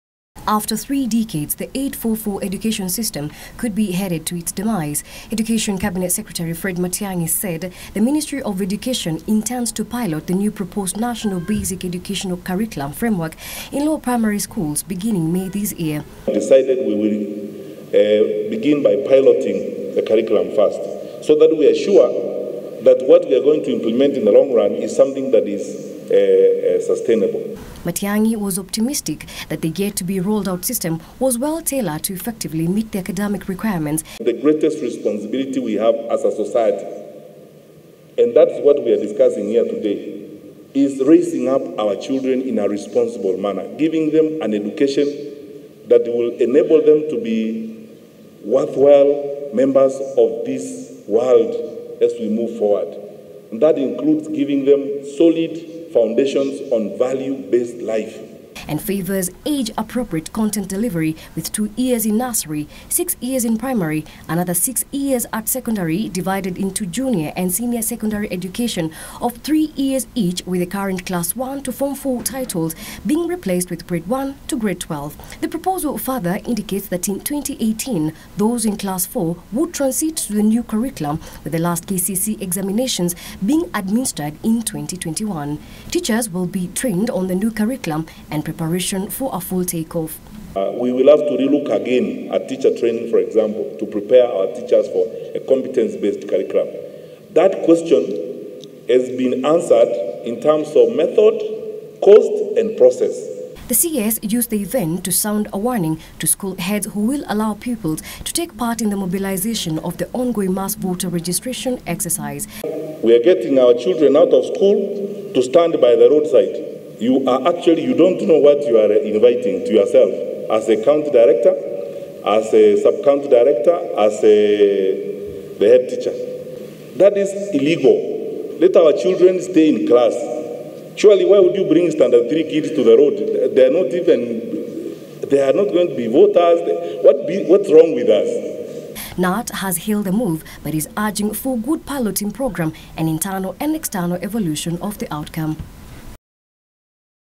8-4-4 education system to be replaced with 2-6-6-3
The ministry of Education intends to pilot the proposed National Basic Education Curriculum Framework in lower primary schools beginning May this year. Addressing stakeholders at the National Conference on Curriculum Reform, Education Cabinet Secretary Fred Matiang’i said the move will enable the country gauge the efficiency of the revised Education system that seeks to replace the 8-4-4 which has been in place for the last 31 years.